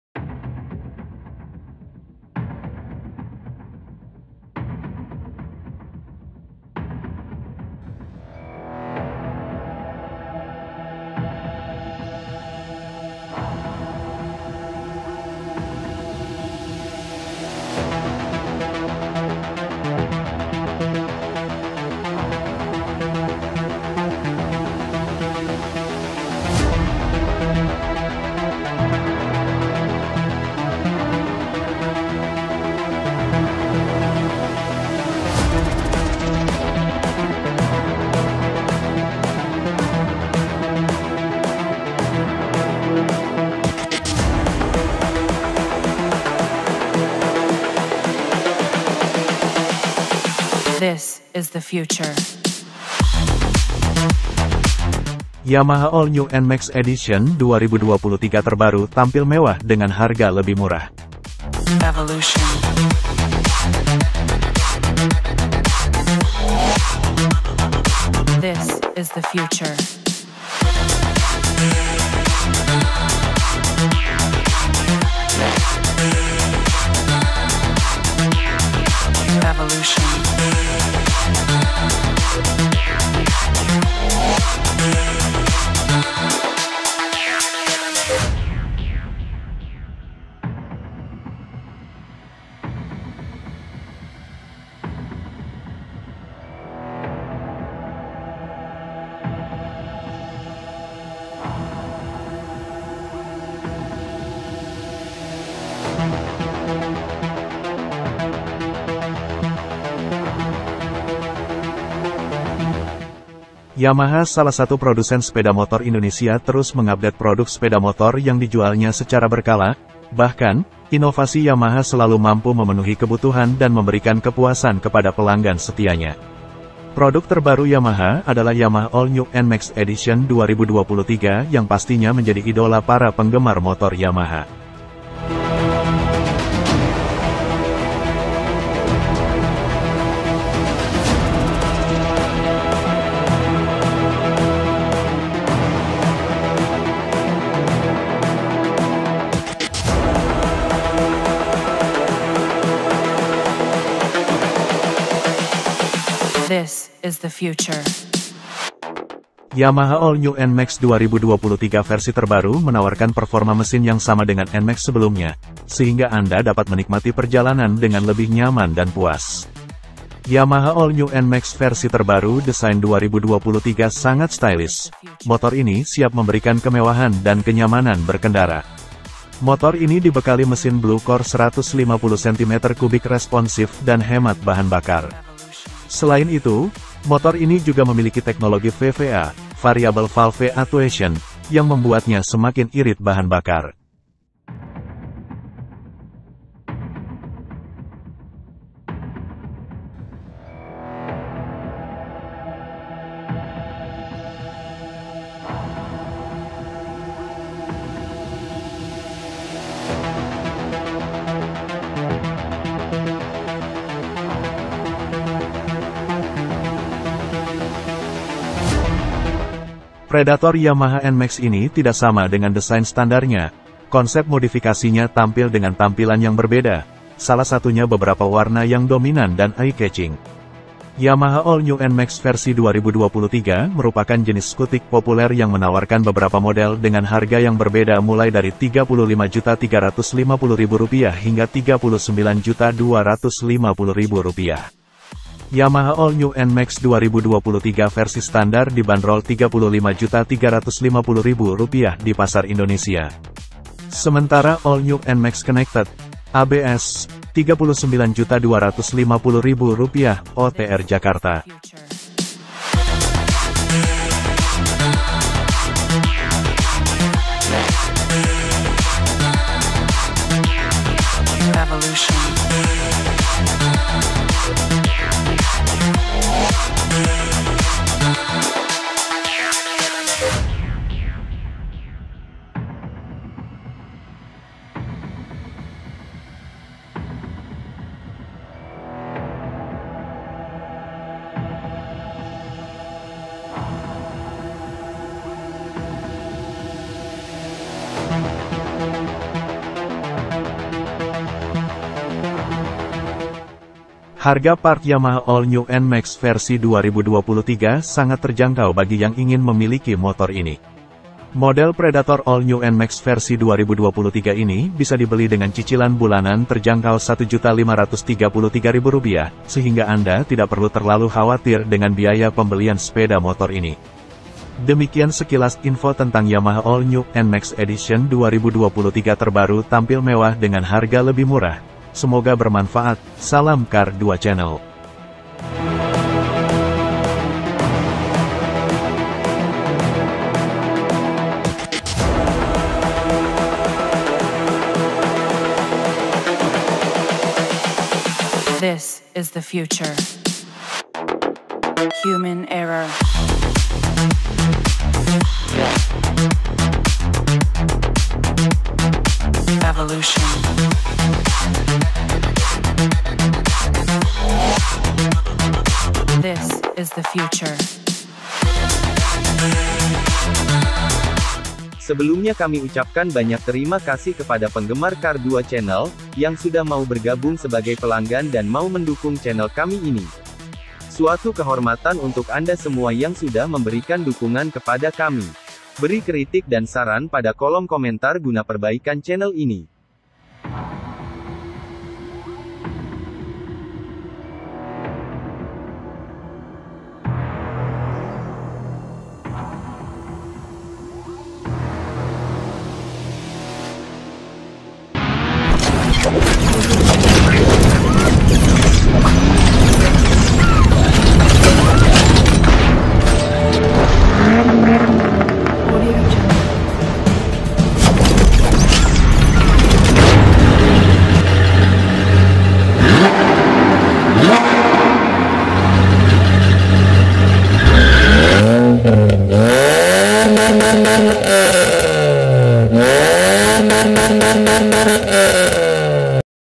NMAX new Yamaha terbaru 2023
NMAX new Yamaha terbaru 2023 tampil mewah dengan harga lebih  murah.
 Yamaha salah satu produsen sepeda motor Indonesia terus mengupdate produk sepeda motor yang dijualnya secara berkala, Bahkan, inovasi Yamaha selalu mampu memenuhi kebutuhan dan memberikan kepuasan kepada pelanggan setianya.

 Produk terbaru Yamaha adalah Yamaha All New NMax Edition 2023 yang pastinya menjadi idola para penggemar motor Yamaha. Yamaha All New NMax 2023 versi terbaru menawarkan performa mesin yang sama dengan Nmax sebelumnya, sehingga Anda dapat menikmati perjalanan dengan lebih nyaman dan puas.

 Yamaha All New NMax versi terbaru desain 2023 sangat stylish, motor ini siap memberikan kemewahan dan kenyamanan berkendara.
 Motor ini dibekali mesin Blue Core 150cc responsif dan hemat bahan bakar.